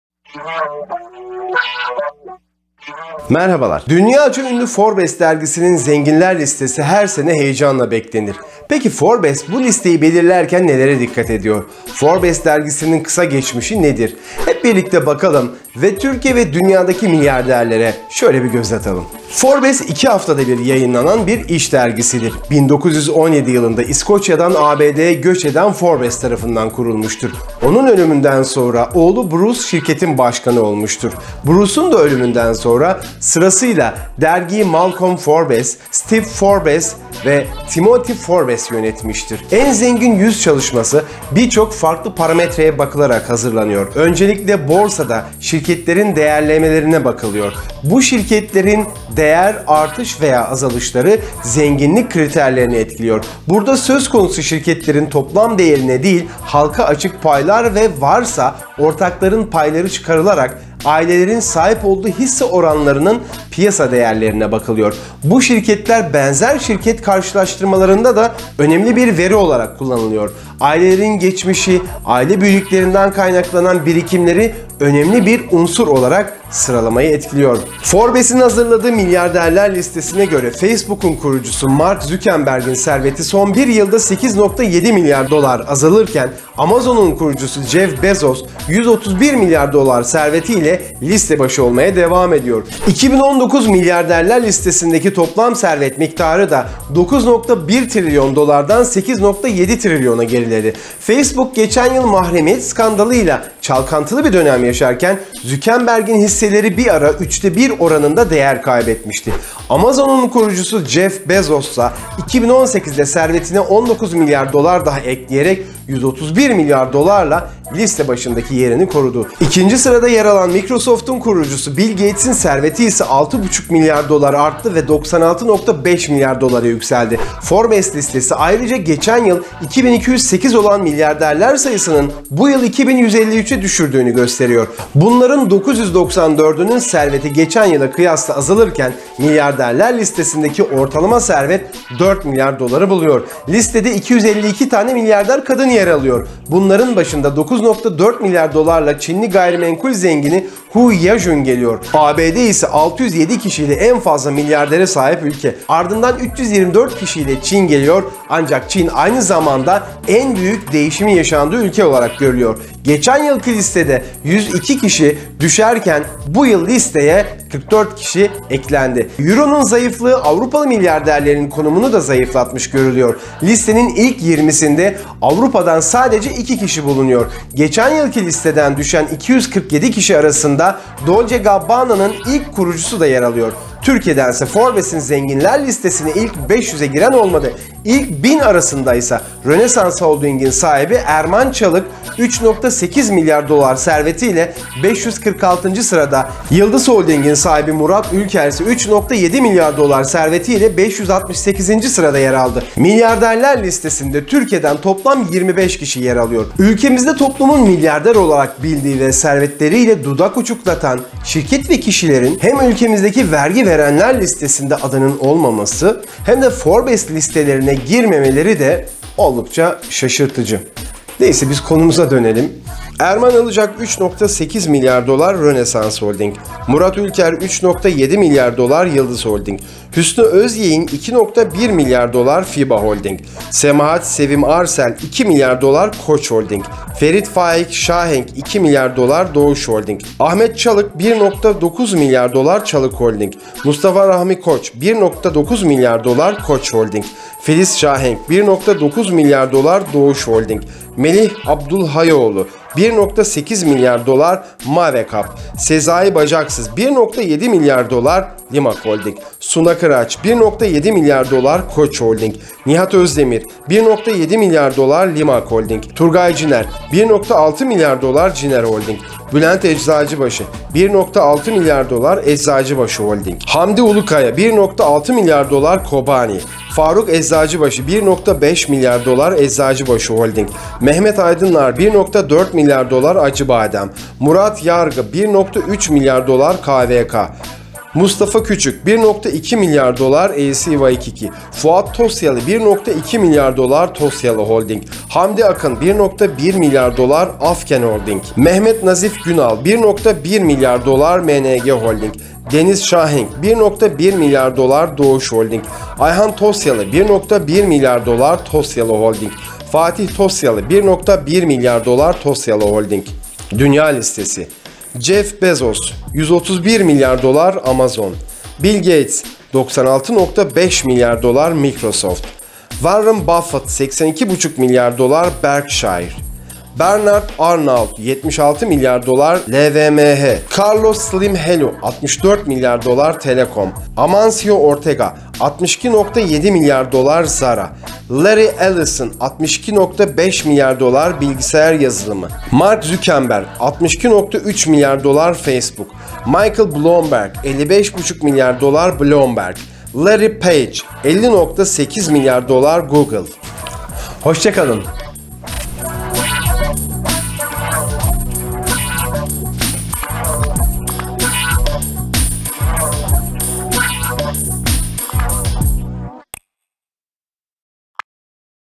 Türkiye'nin ve Dünyanın En Zenginleri Kimdir? Forbes 2019 Yılın Zenginlerini Açıkladı!
forbes2019 zenginler zengininsanlar Türkiye'nin ve Dünyanın En Zenginleri Kimdir? Forbes 2019 Yılın Zenginlerini Açıkladı!

Dünyaca ünlü Forbes Dergisinin zenginler listesi her sene heyecanla beklenir. Peki Forbes, bu listeyi belirlerken nelere dikkat ediyor, Forbes dergisinin kısa geçmişi nedir, hep birlikte inceleyelim, Türkiye ve dünyadaki milyarderlere bakalım.

En Zengin 100 çalışması birçok farklı parametreye bakılarak hazırlanıyor. Öncelikle borsada şirketlerin değerlemelerine bakılıyor.

Forbes'un hazırladığı milyarderler listesine göre, Facebook'un kurucusu Mark Zuckerberg'in serveti son bir yılda 8,7 milyar dolar azalırken, Amazon'un kurucusu Jeff Bezos 131 milyar dolar servetiyle liste başı olmaya devam ediyor.